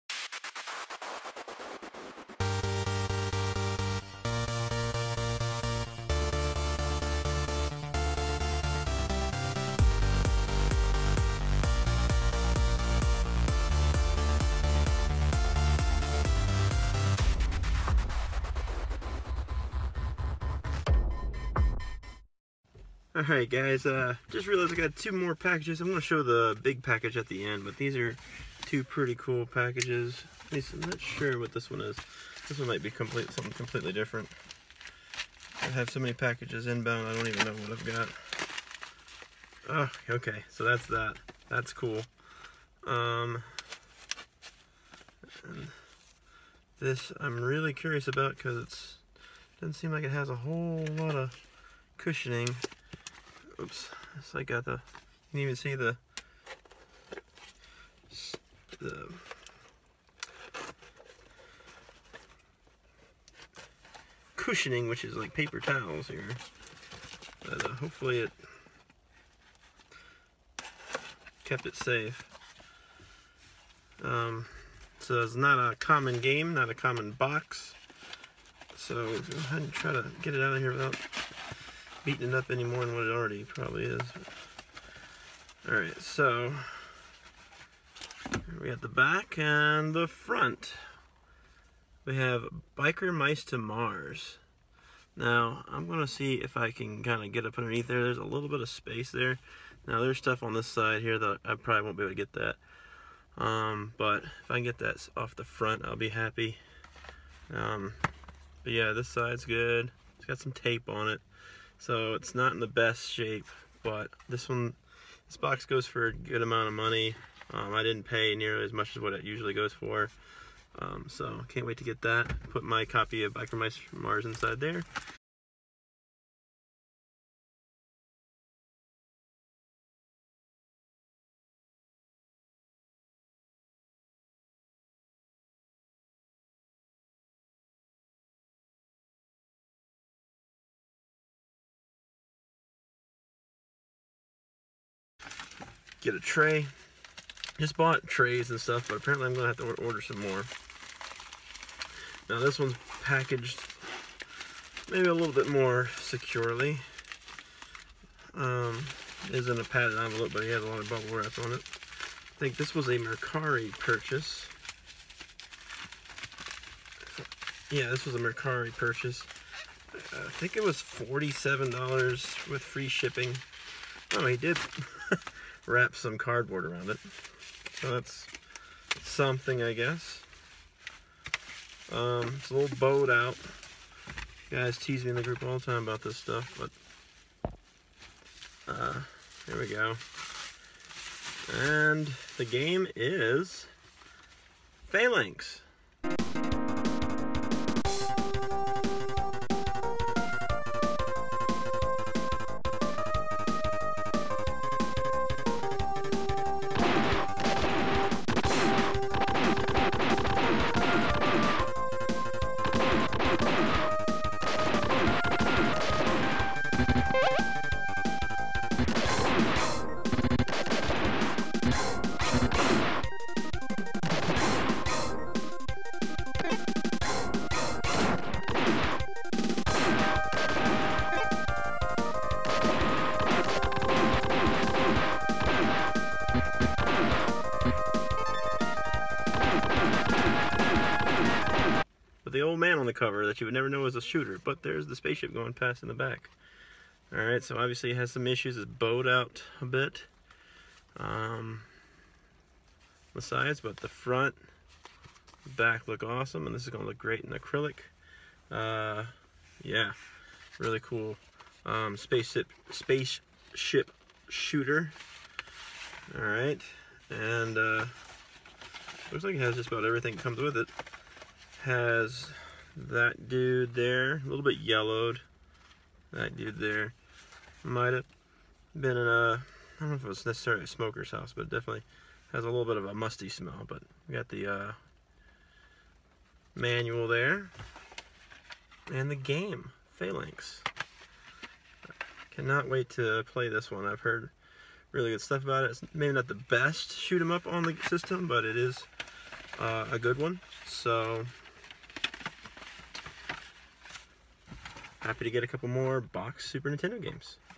A Package For 3P "Biker Mice From Mars and Phalanx"
This episode, I grab a couple deals from eBay and Mercari including an uncommon SNES box covered in stickers (will I be able to clean them off?) and a beloved SNES shooter.